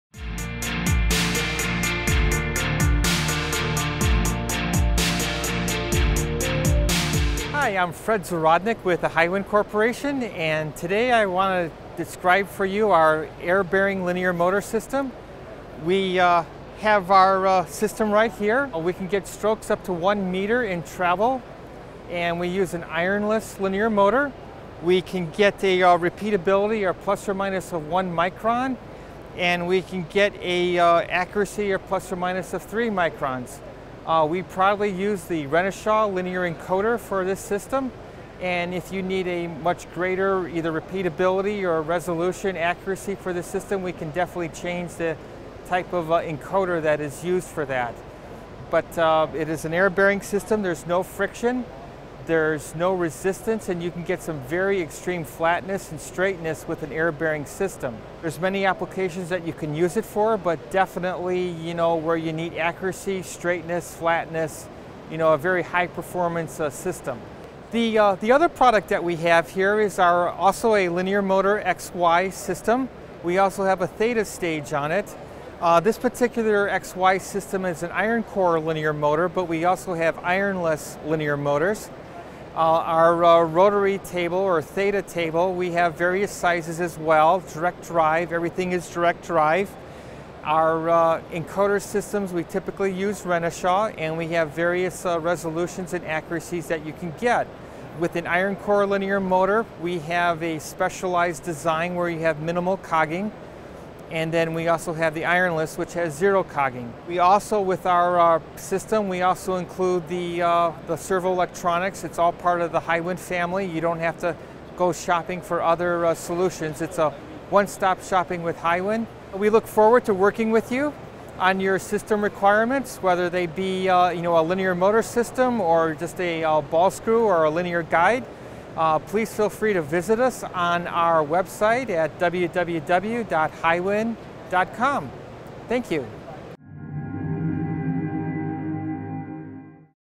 A closer look at air bearing linear motors
Hiwin offers a range of air bearing linear motors and linear motor XY systems. These systems can ironless or not, with minimal or zero cogging. Shown at the 2014 Pack Expo show are air bearing linear motors with 1 m travel strokes, repeatability of +/- 1 micron, accuracy of +/- 3 microns. Air bearing linear motors suit applications needing extreme flatness and straightness.